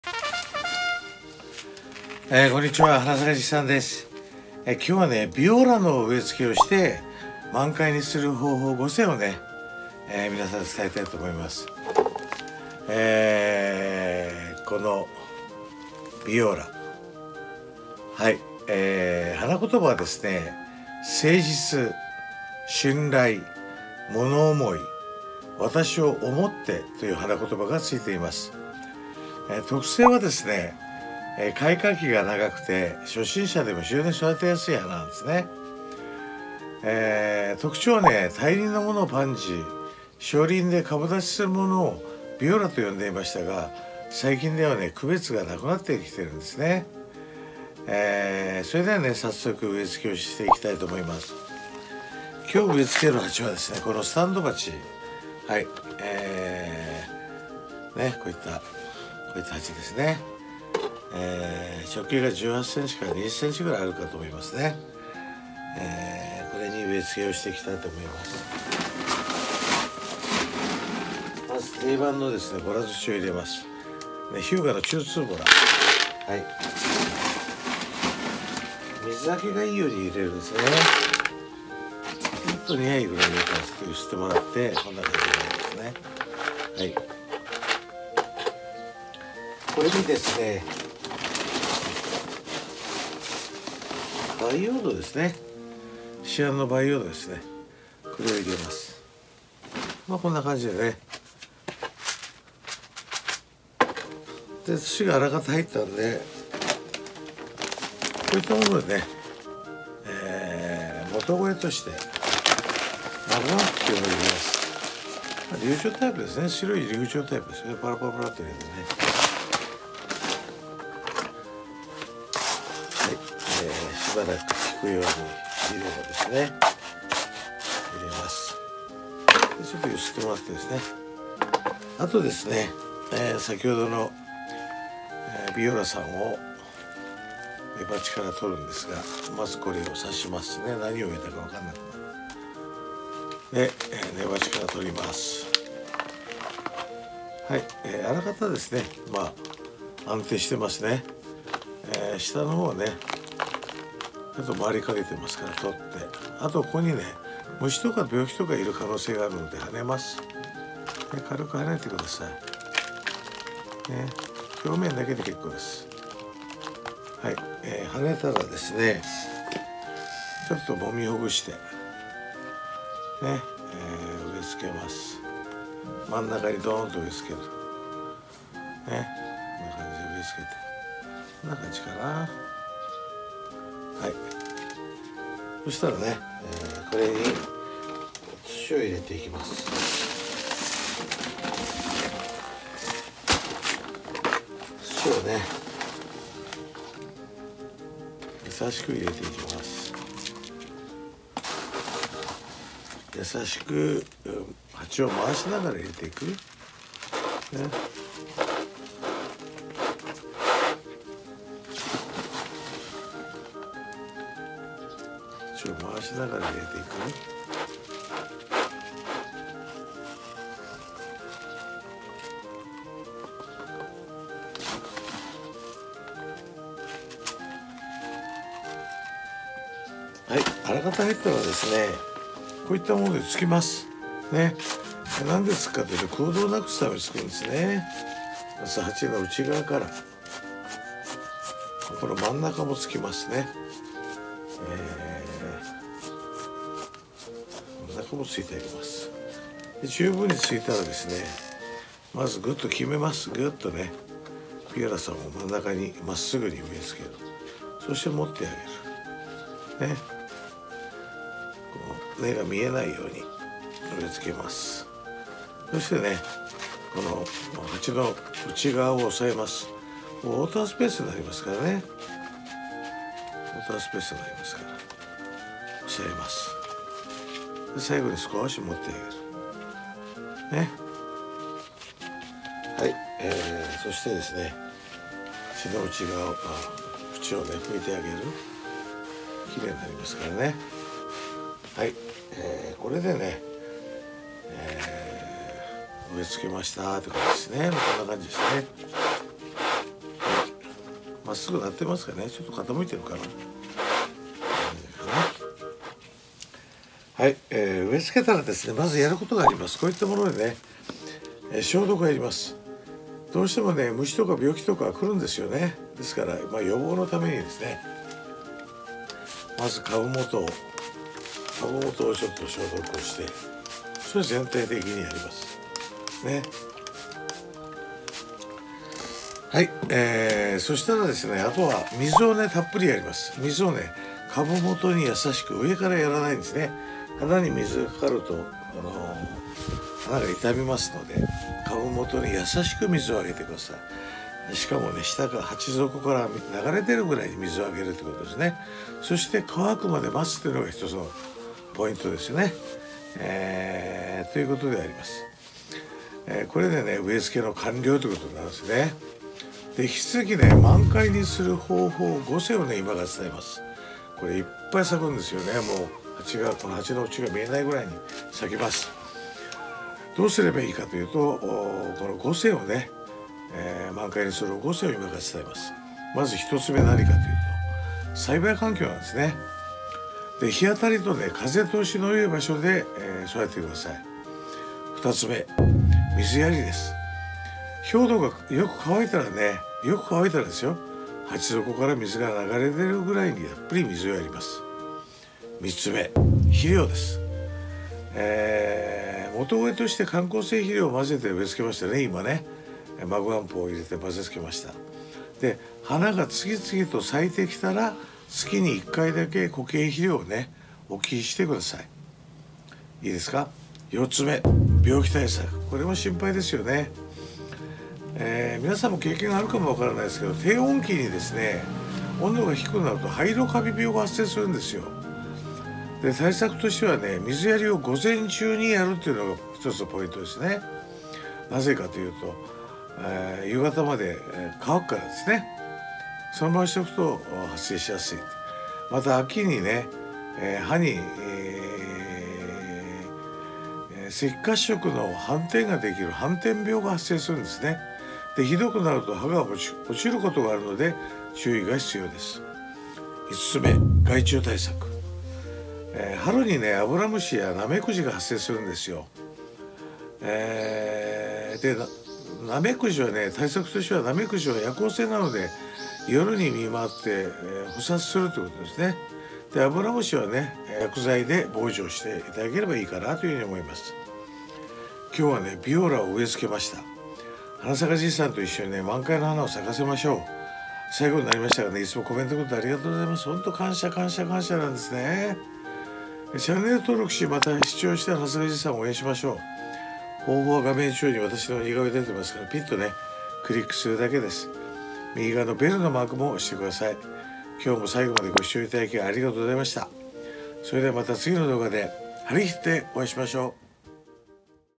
【花】ビオラの育て方・植え付け
初心者の方にわかりやすくビオラを実際に植え付けて
花を満開にする方法5選を伝えます。

【花の説明】
原産地は、ヨーロッパです。
ビオラは、スミレ科・スミレ属で一年草です。
花色は、ホワイトとレッドとピンクとオレンジとイエローとブルーとパープルとブラウンとブラックと複色の10色です。
開花期は、10月下旬から5月中旬です。
特性は、開花期が長く、初心者でも育てやすい花です。
特徴は、大輪のものをパンジー、小輪で株立ちになるものをビオラと呼んでいましたが、最近では区別ができなくなっています。

【関連動画】
①徒長したビオラやパンジーの切り戻し方テクニックを伝授！ただ切ればいいだけじゃないのです?！